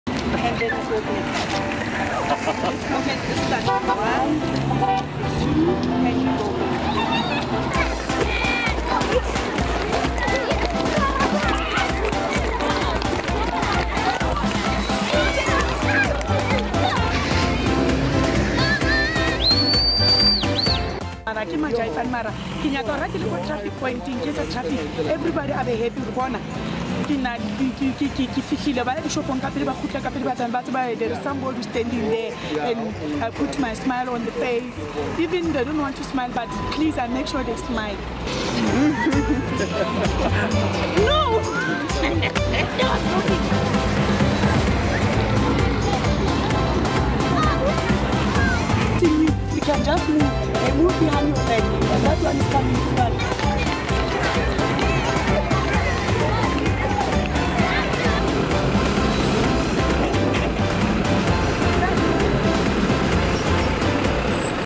Meet Dorah, the viral dancing traffic cop, teaching motorists to smile
Meet Dorah Mafokeng, the South African traffic officer who went viral after a video was published showing her awesome traffic directing skills. She is now teaching others her moves too!